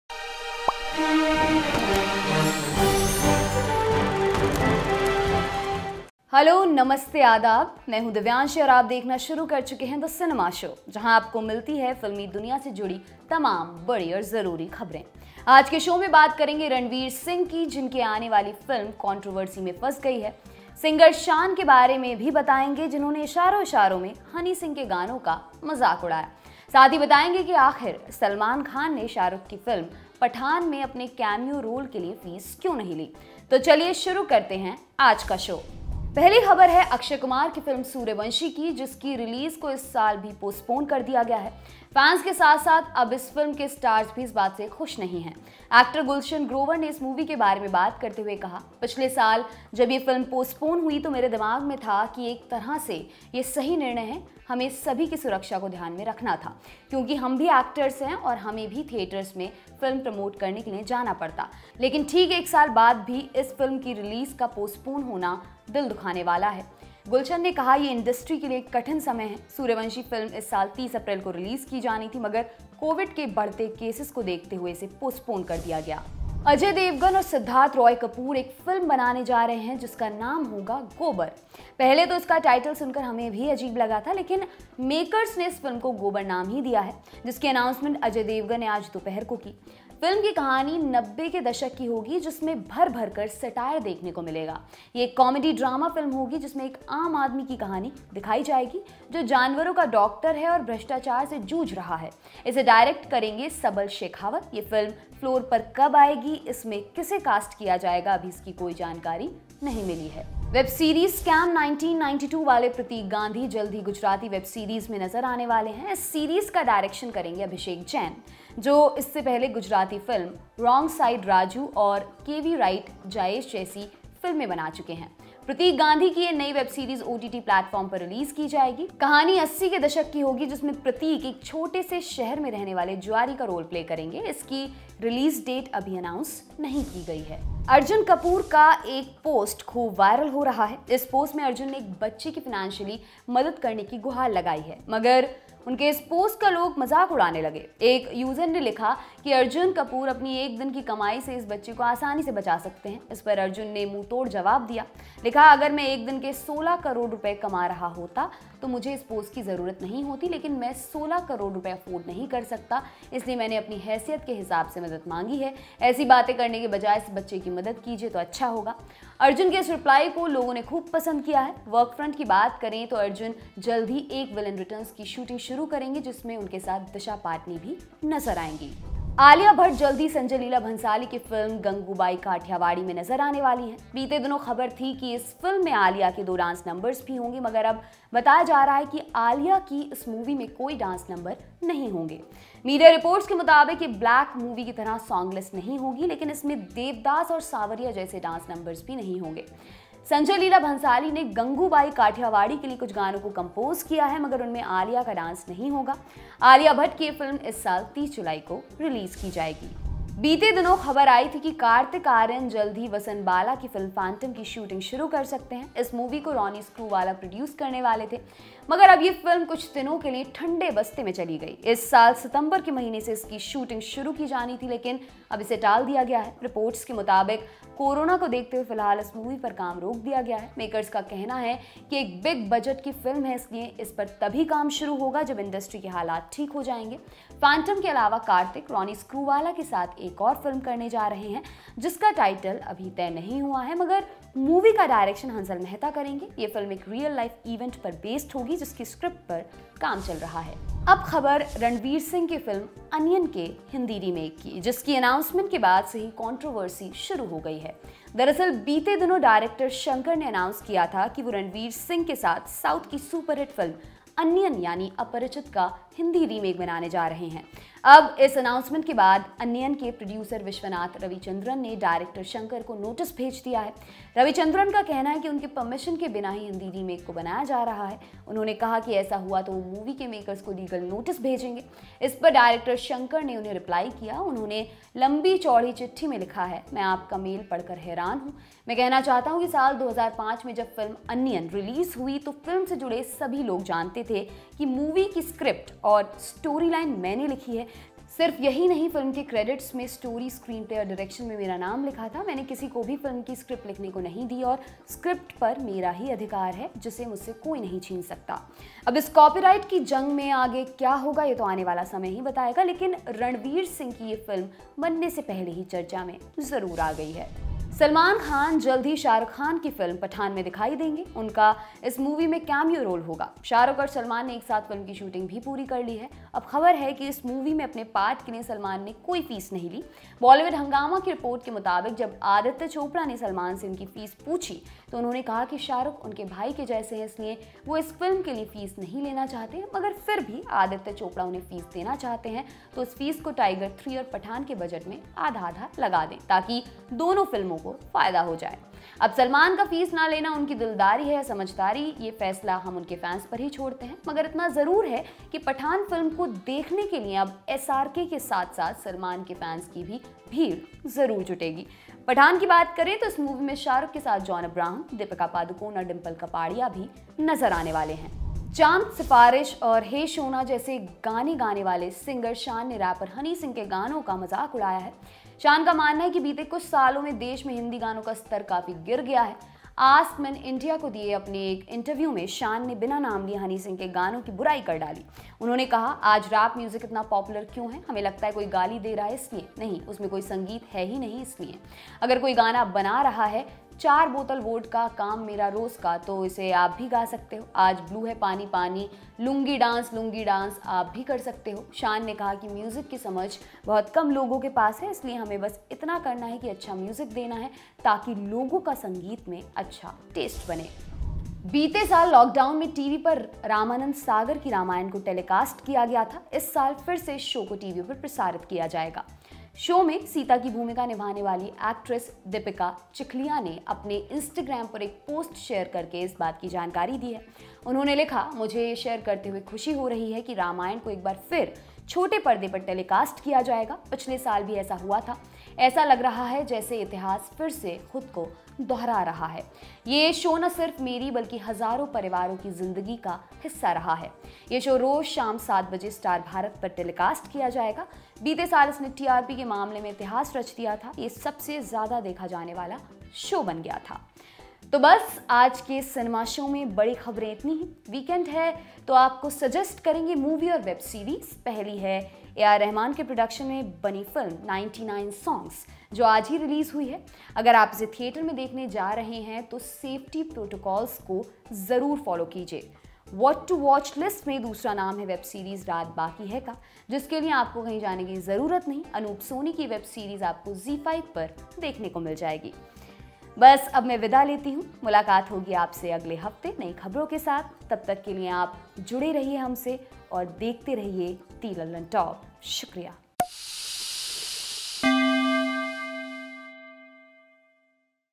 Singer Shaan ने इशारों-इशारों में Honey Singh के फेमस गानों पर क्या तंज़ कस दिया? I The Cinema Show
The Cinema Show, E169 - 16 April, 2021

In Today's Cinema Show we will give you the latest Bollywood updates, TV, Film and International cinema news. Gulshan Grover says on Akshay Kumar's Sooryavanshi release date, Arjun Kapoor slammed troll who took a dig after he shared a post asking to donate for the treatment of a child, Pratik Gandhi’s play a gambler in his next Gujarati web series, Alia Bhatt's Gangubai Kathiawadi has no dance numbers, Kartik Aaryan's Phantom delayed, Shankar responds to allegations made by Anniyan producer against Hindi remake with Ranveer Singh, Salman Khan refuses to take fee for his role in Shah Rukh Khan’s Pathan, Shaan take a dig at rapper Yo Yo Honey Singh's songs, Ramanand Sagar's Ramayan will air once again and Ajay Devgn has produce movie with Siddharth Roy Kapur titled Gobar.